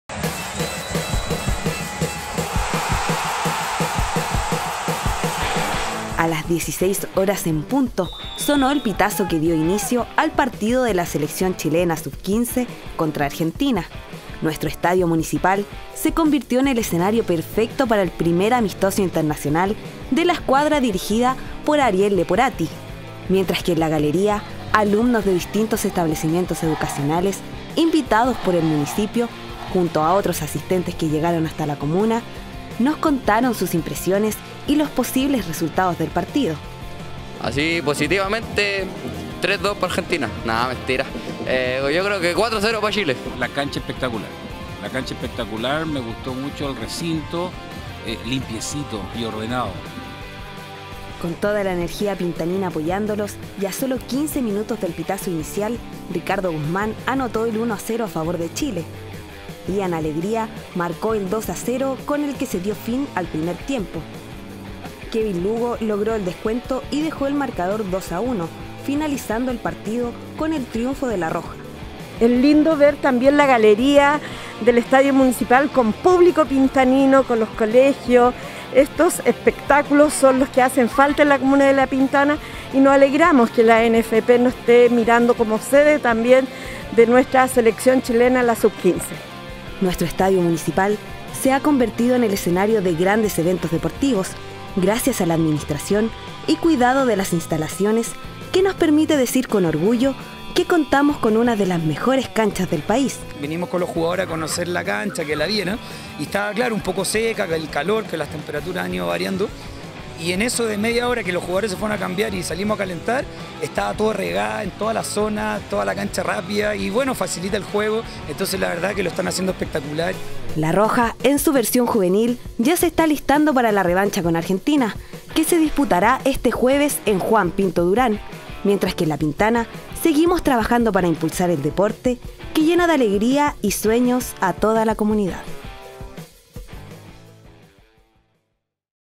La Pintana vivió una jornada histórica en Estadio Municipal
La selección de Chile Sub 15 le ganó a la Argentina Futuras promesas del balompié nacional y un público muy entusiasta vivieron una jornada histórica porque nuestros futuros cracks de la Selección Chilena Sub 15 le ganaron a su par de la Selección Argentina en nuestro Estadio Municipal.Un partido de campanillas en el que los asistentes pudieron disfrutar de un triunfo ante el conjunto trasandino 2 por 1.